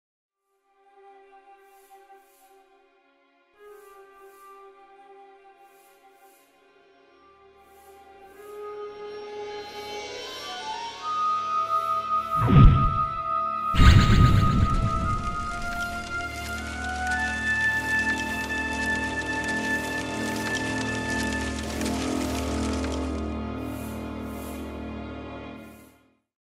Flutter Bombs :: Daisy
Introducing another member of Flutter Bombs: Daisy.  Named after the BLU-82, also known as a 'Daisy Cutter', this weapon instantly hits combatants in its radius with fire damage, followed up with a brief damage over time effect.